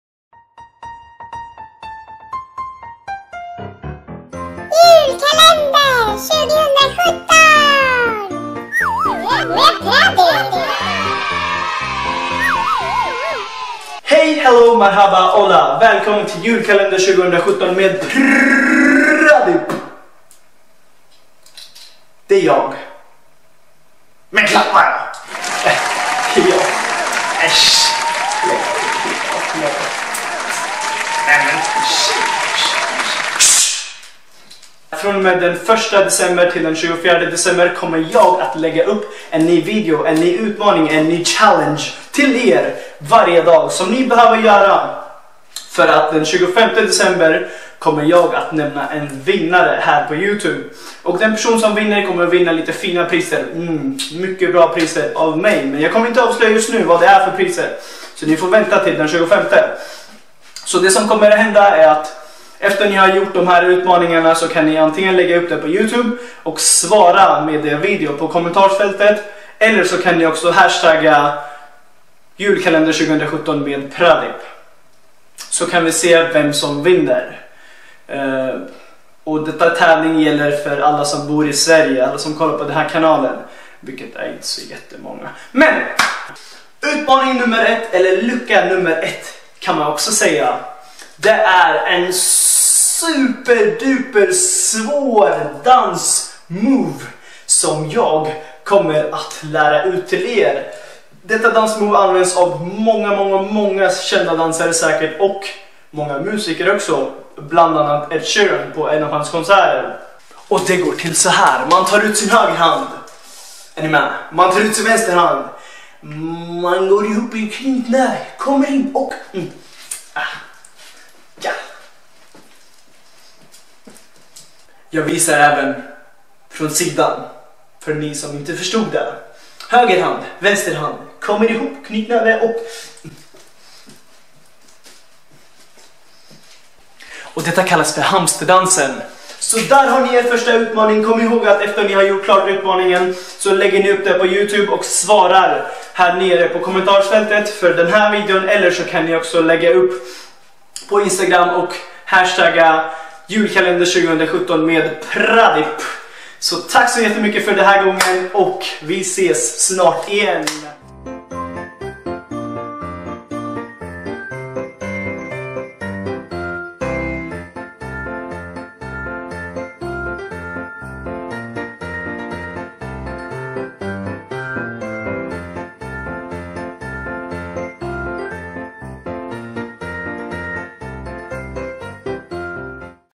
JULKALENDER 2017 MED PRADIP - Lucka 1
Hej hallå :D

Detta är Lucka nr 1 för min Julkalender 2017 :D Hoppas ni gillar det:D

Detta är mina utmaningar/challenges till er.. Varje dag fram till 24 dec kommer det en ny utmaning som jag hoppas ni gör :D

Varför? För att ni kommer att vinna fina priser!!
Vad för priser? FINA priser ;)

Vad behöver ni göra?
Ni behöver filma er själva när ni gör de här challenges och lägga upp det på youtube och svara på kommentarsfältet för den videon som ni gör challenge till eller ni kan lägga upp det på instagram och hashtagga

Vilka får vara med?
ALLA SOM VILL :) Inga åldersgränser :D

Lycka till och hoppas jag får några svar av er ;)